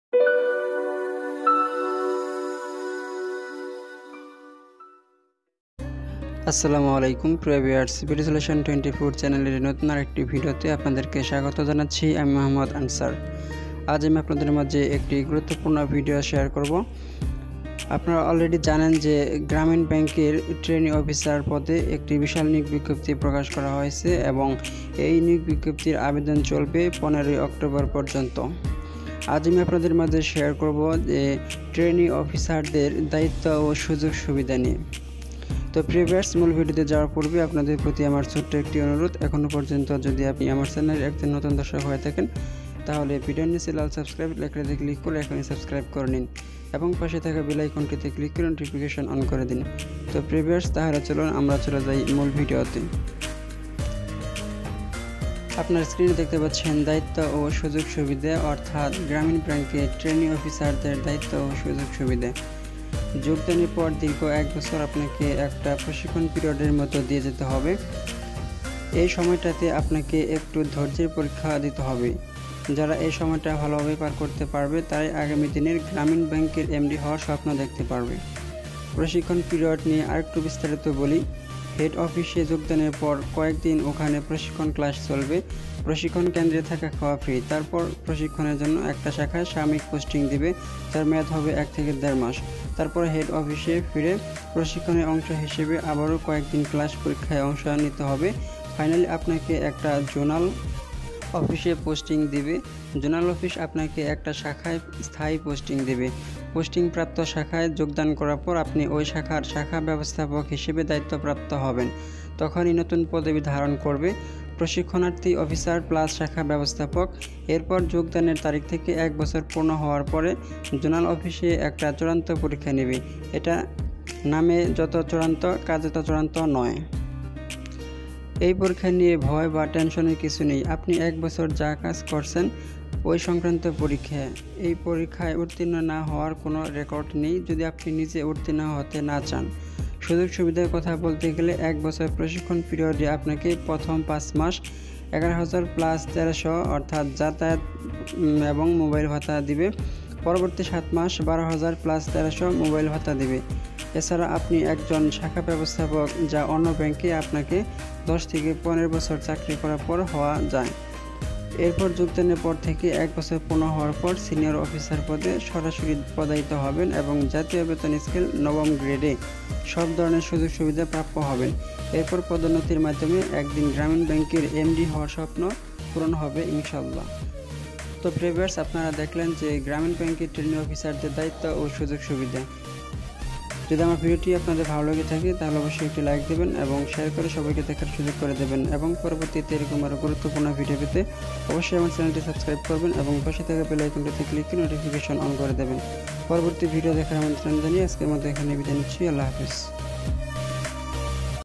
গ্রামীণ ব্যাংকের ট্রেইনী অফিসারদের দায়িত্ব ও সুযোগ সুবিধা | Grameen bank trainee officer
In this video I discussed about the responsibility and opportunities of trainee officer in Grameen Bank.